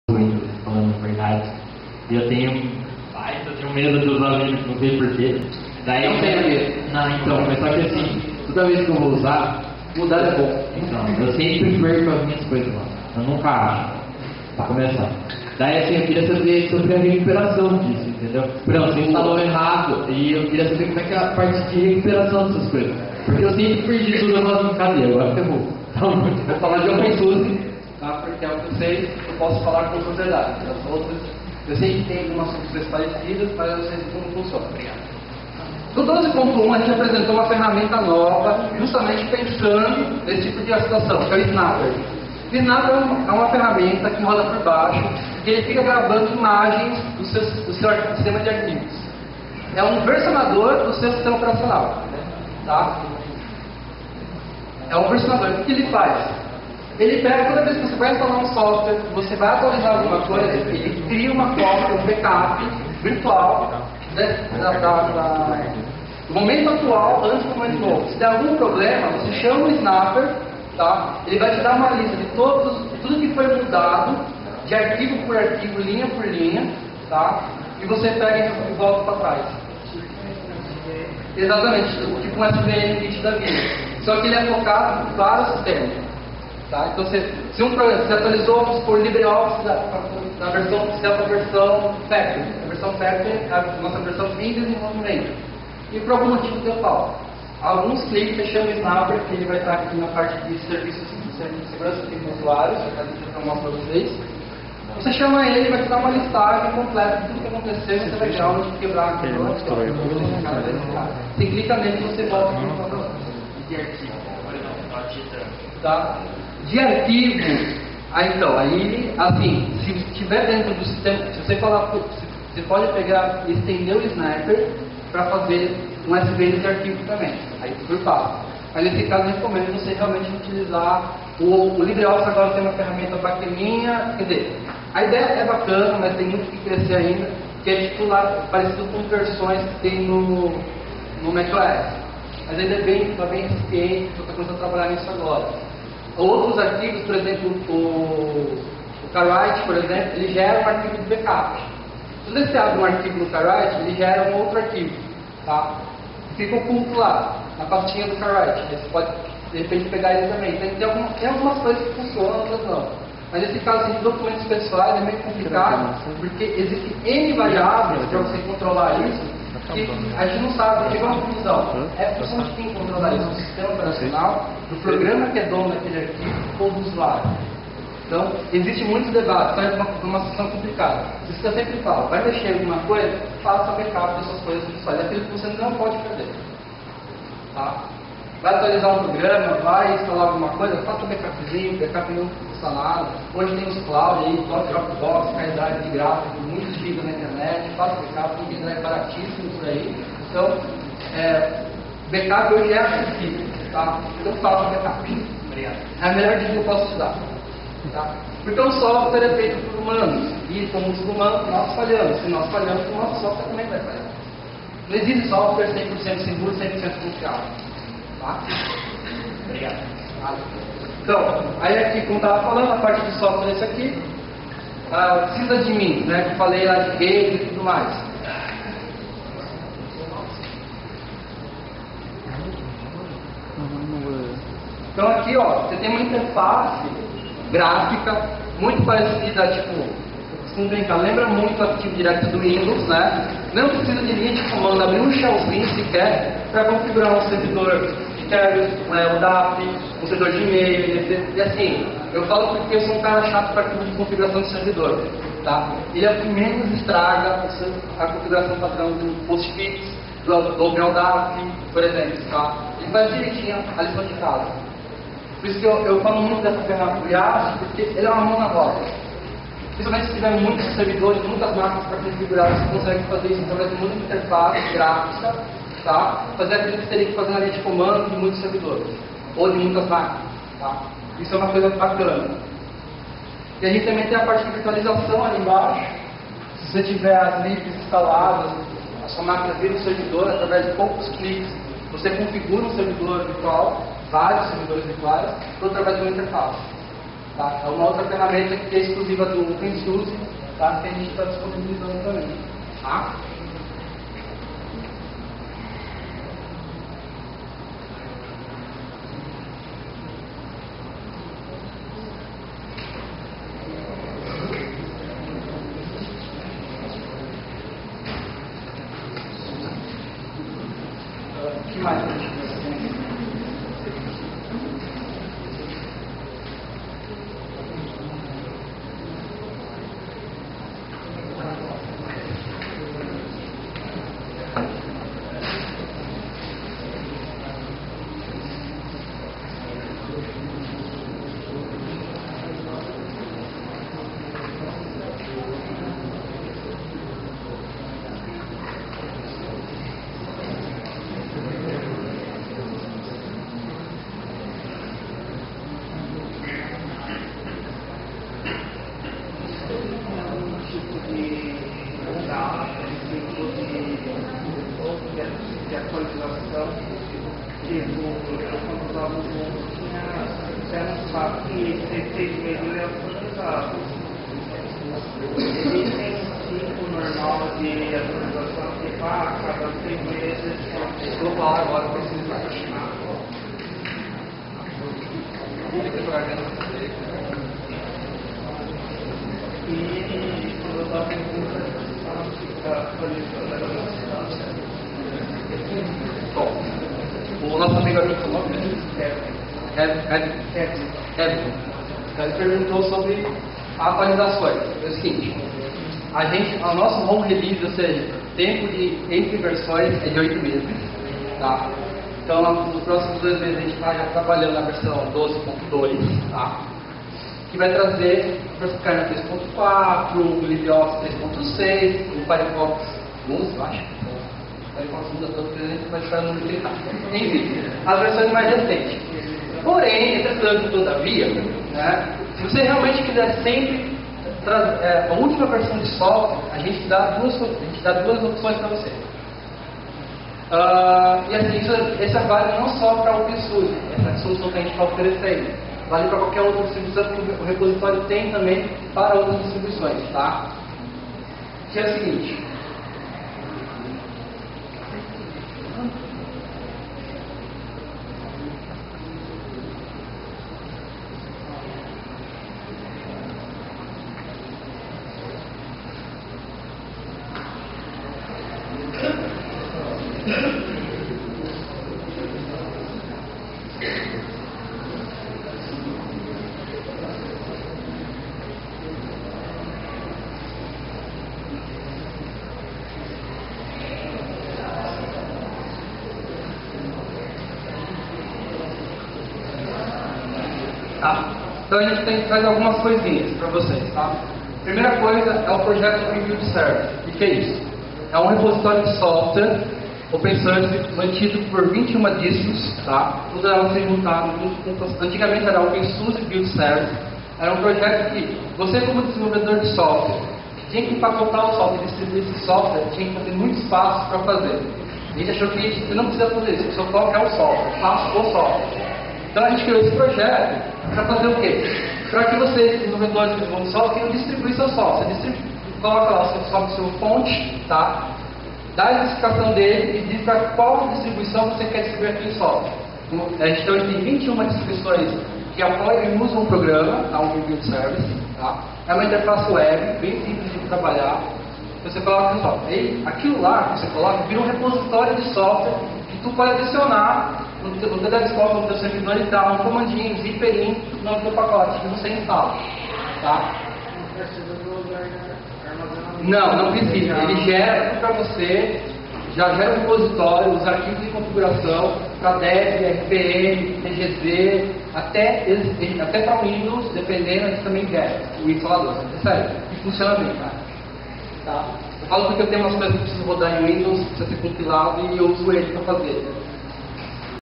Flisol Indaiatuba 2012 - Palestra: OpenSUSE 12, um novo ar nos desktops e servidores - Parte 3
Palestra: OpenSUSE 12, um novo ar nos desktops e servidores - Parte 3

Palestrante: Domingos Teruel - Tecnólogo em Gestão de TI, especialista em interfaces de sistemas, atuando no desenvolvimento e implementação de sistemas web com software livre. Membro ativo das comunidades locais de KDE, PHP, Zend Framework e Mandriva. Contribui com os projetos de marketing do openSuSe e KDE.

Conteúdo: O Projeto openSUSE apresenta o release 12 do openSUSE. Este release inclui as versões mais recentes dos ambientes GNOME, KDE, LibreOffice, Firefox e o Kernel Linux e muito mais. Na versão 12 estão disponíveis mais de 1000 aplicações open source. openSUSE também inclui uma completa suite de software para servidores e uma rica seleção de ferramentas de desenvolvimento open source como o OpenBuildService e SuSeStudio.